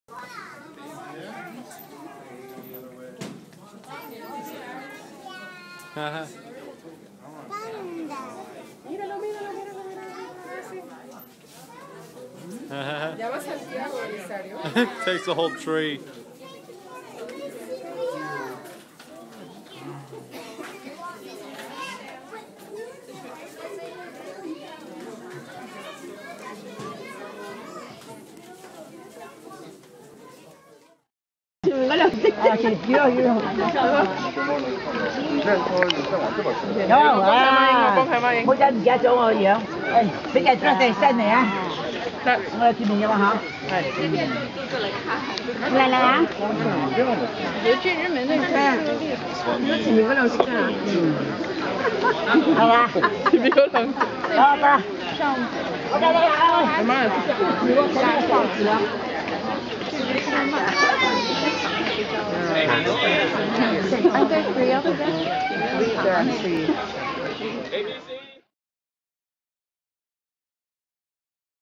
Panda bear eats a tree: pandas have a voracious appetite
The panda bear or giant panda (ailuropoda melanoleuca), known in its native China as "大熊猫/dà xióng māo" ("big bear cat"), is an adorable animal that has captured the hearts of people the world over and is in hot demand among zoos.

The giant panda has a carnivorous digestive system but does not eat meat. As such, they must eat a lot of bamboo to survive. Look at this one: he takes a whole tree to eat! A visitor expresses his surprise at this hearty appetite. Bonus footage shows baby pandas eating bamboo.